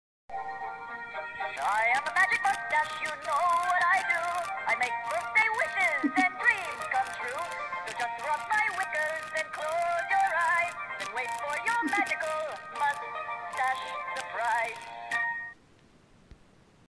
Magic Mustache Singing Birthday Card
Randall got this for me on my 2015 bday. Very silly!